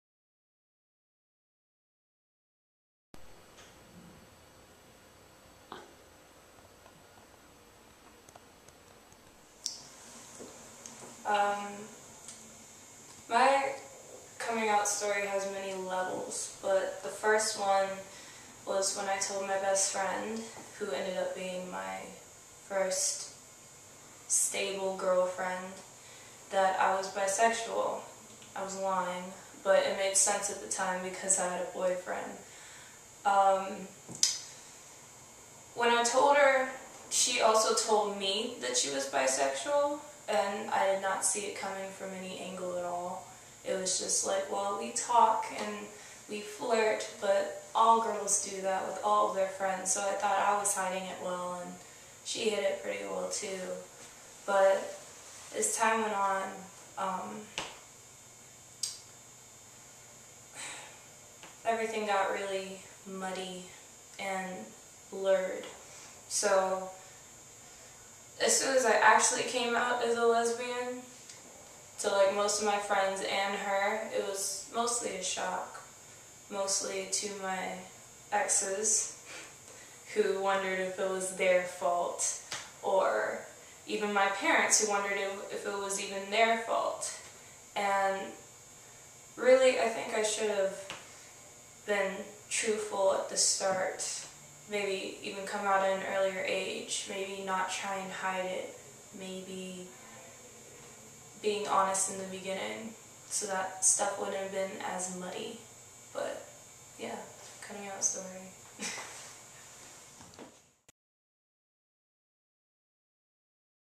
Coming Out: Story 8
Here is a story about coming out to friends and finding you the feelings reciprocated.  Another part of SMYAL's series of stories for National Coming Out Day 2012!

Check out more stories on our YouTube page, and learn more about SMYAL